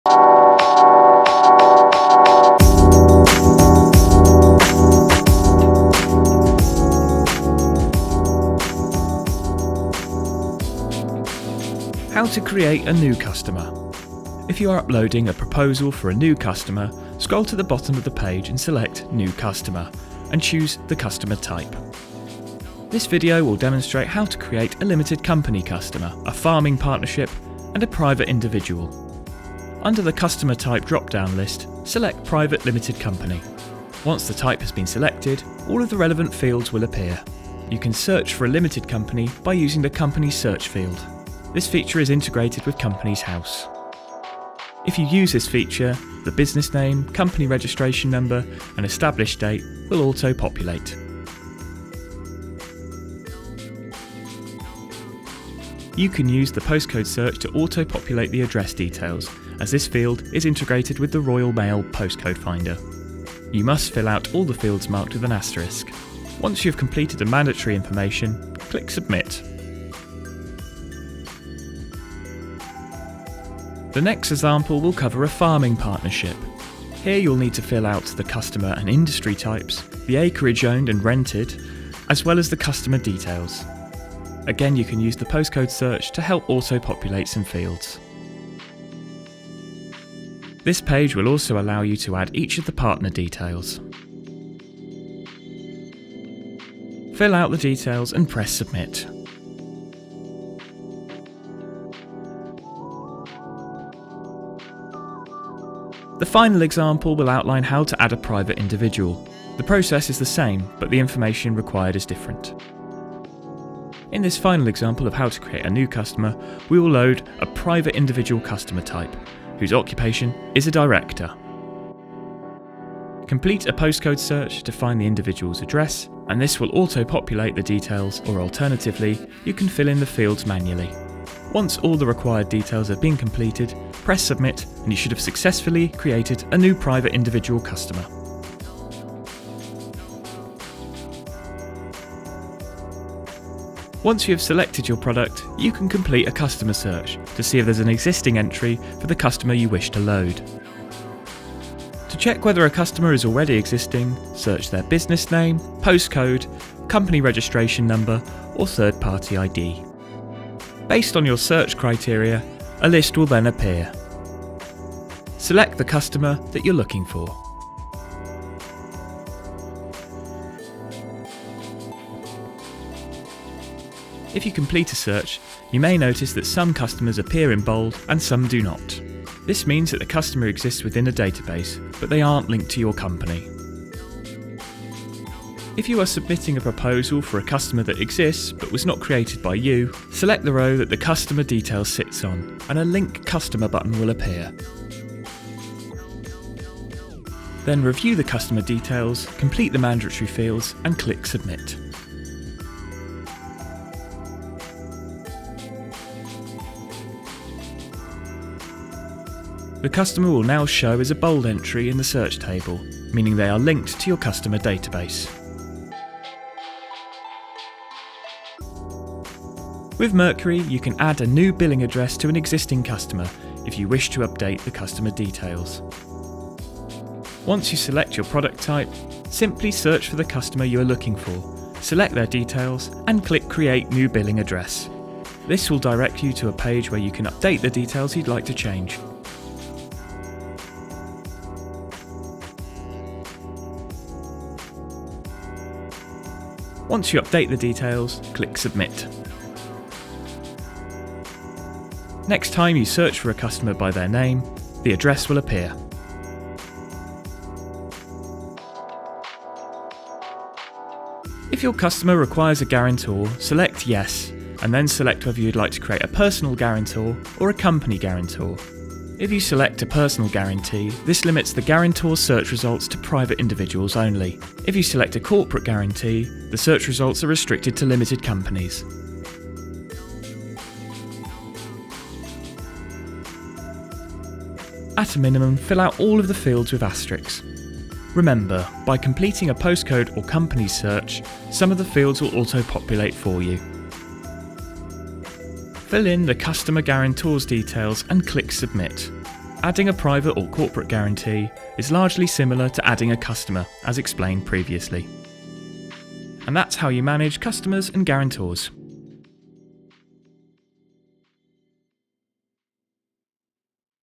Managing customers and guarantors | Novuna Business Finance
How to add a new customer, search a database of existing customers, add a new billing address for an existing customer,  link customers to your database and optionally add a guarantor on Mercury